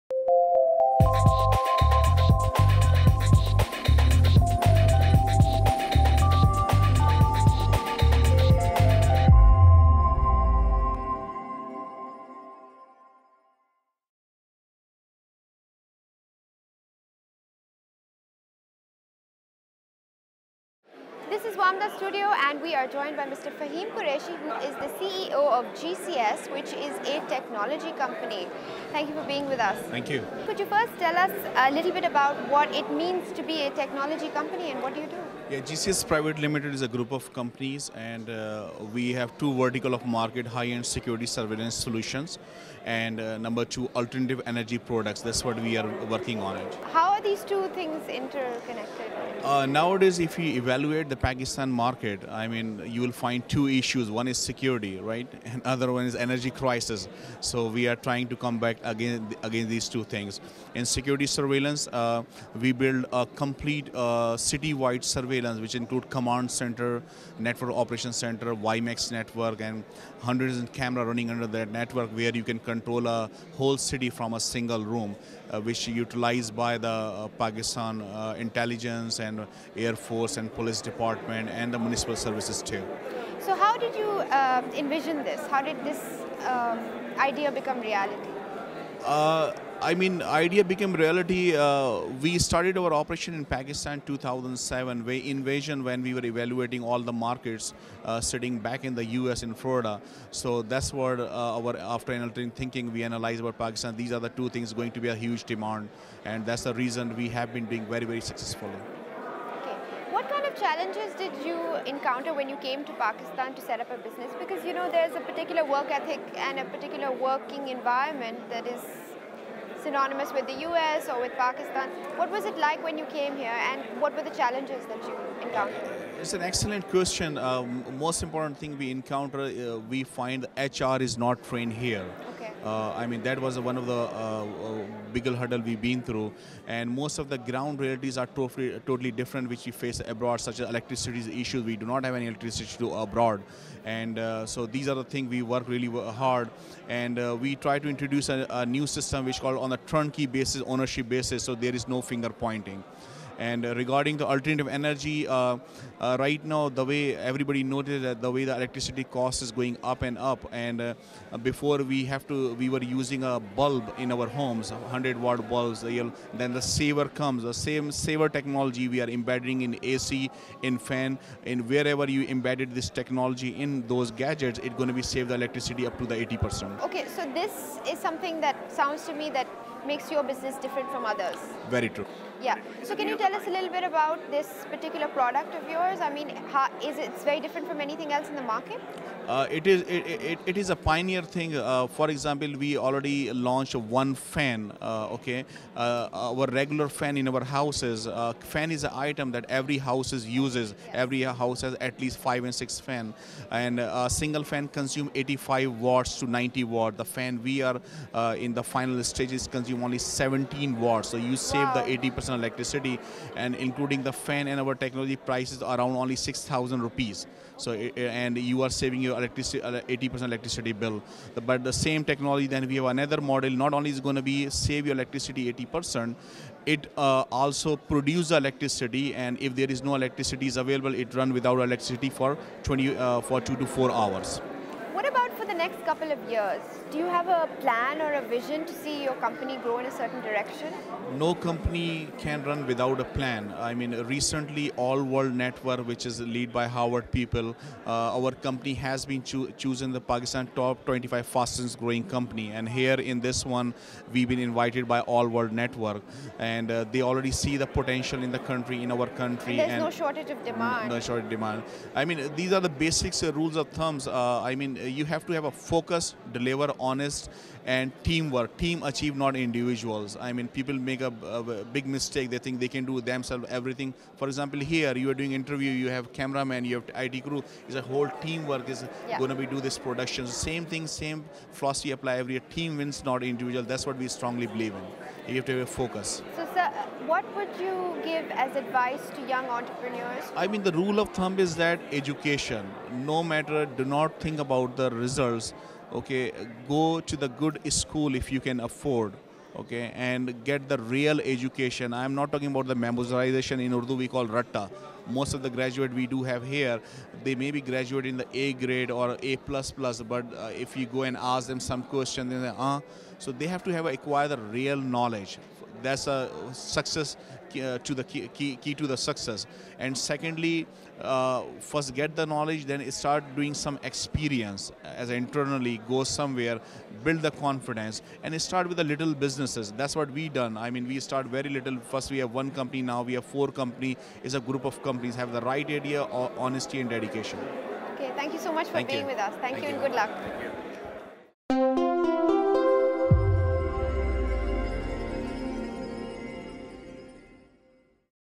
Advice for Building a Fast-Growth Company
Faheem Qureshi, the CEO of Karachi-based tech company GCS, discusses how his company, which was listed in the Pakistan Fast Growth 25 by the AllWorld Network, is working to address security challenges in Pakistan and create alternative energy solutions. When it comes to building a company, Qurashi emphasizes both the creation of a good team and the importance of education and experience, and starting small.